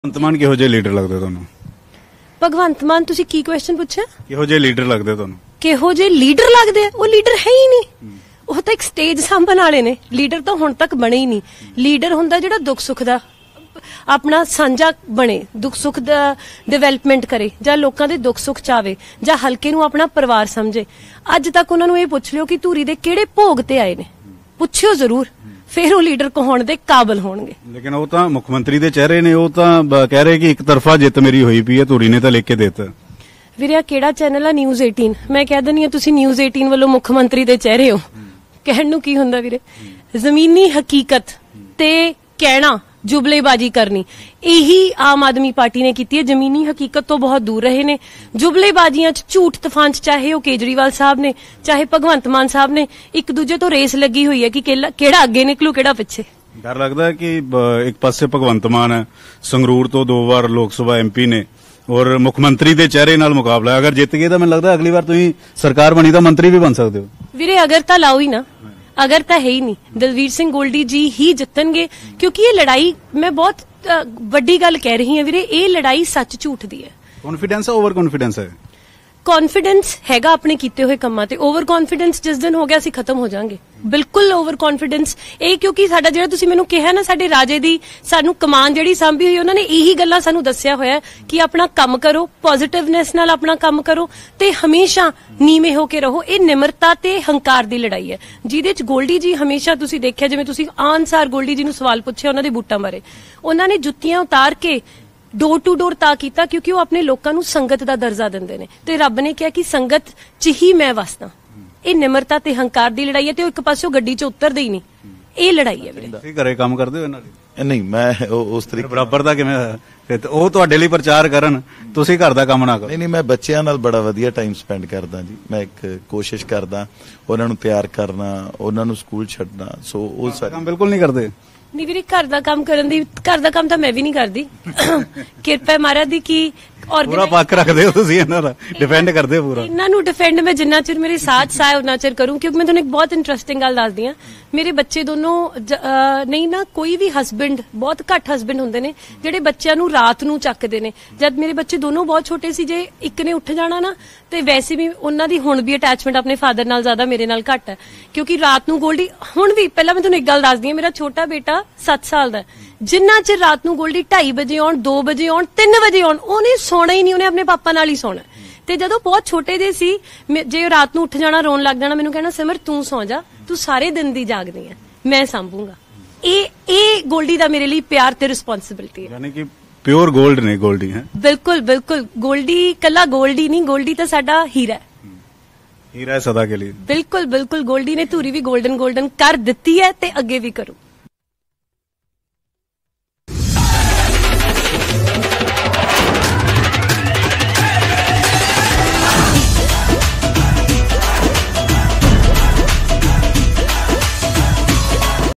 Punjab Election 2022 : 'Bhagwant Mann ਲੀਡਰ ਨਹੀਂ ਸਟੇਜ ਸਾਂਭਣ ਵਾਲੇ' | Goldy Wife | News18 Punjab
Dalvir Goldy Live : ਹੌਟ ਸੀਟ Dhuri ਧੂਰੀ ਤੇ Congress ਕਾਂਗਰਸ ਉਮੀਦਵਾਰ Dalvir Goldy ਦਲਵੀਰ ਗੋਲਡੀ ਨੇ ਪੂਰੀ ਵਾਹ ਲਗਾਈ ਹੋਈ ਹੈ। ਗੋਲਡੀ ਦਾ ਸਿੱਧਾ ਮੁਕਾਬਲਾ AAP ਦੇ CM ਉਮੀਦਵਾਰ ਭਗਵੰਤ ਮਾਨ ਨਾਲ ਹੈ। ਗੋਲਡੀ ਨੇ ਨਿਊਜ਼18 ਨਾਲ ਖਾਸ ਗੱਲਬਾਤ ਚ ਦੱਸਿਆ ਕਿ ਉਹ ਕਿੰਨ੍ਹਾ ਮੁੱਦਿਆਂ ਤੇ ਚੋਣ ਲੜ ਰਹੇ ਨੇ। Dalvir Goldy ਦੀ ਪਤਨੀ ਨੇ ਭਗਵੰਤ ਮਾਨ ਤੇ ਨਿਸ਼ਾਨਾ ਸਾਧਿਆ ਤੇ ਕਿਹਾ ਕਿ ਭਗਵੰਤ ਮਾਨ ਕੋਈ ਲੀਡਰ ਨਹੀਂ ਹੈ ਉਹ ਸਟੇਜ ਸਾਂਭਣ ਵਾਲੇ ਨੇ। ਉਨ੍ਹਾਂ ਕਿਹਾ ਕਿ ਉਹ ਧੂਰੀ ਚ ਕਦੇ ਕਿਸੇ ਦੇ ਦੁੱਖ- ਸੁੱਖ ਚ ਸ਼ਾਮਿਲ ਨਹੀਂ ਹੋਏ। ਉਨ੍ਹਾਂ ਕਿਹਾ ਕਿ ਆਪ ਝੂਠੀ ਪਾਰਟੀ ਹੈ।